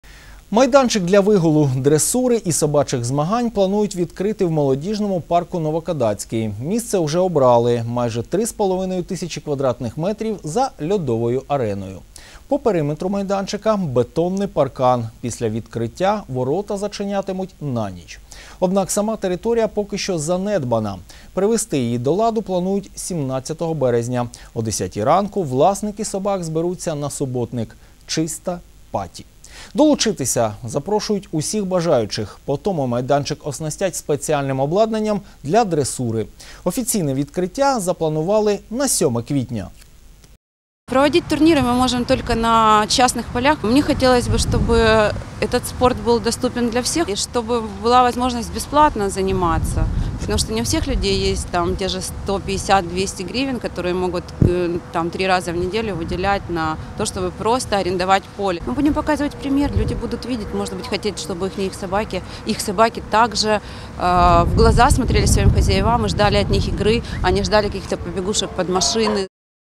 Перший муніципальний майданчик для вигулу і дресури собак відкриють у Дніпрі
Майданчик для вигулу, дресури і собачих змагань планують відкрити в Молодіжному парку “Новокодацький”

Місце уже обрали - майже три з половиною тисячі квадратних метрів за льодовою ареною. По периметру майданчика - бетонний паркан, після відкриття ворота зачинятимуть на ніч. Однак сама територія поки що занедбана. Привести її до ладу планують 17 березня. О десятій ранку власники собак зберуться на суботник “Чисто Паті”. Долучитись запрошують усіх бажаючих. По тому майданчик оснастять спеціальним обладнанням для дресури. Офіційне відкриття запланували на 7 квітня.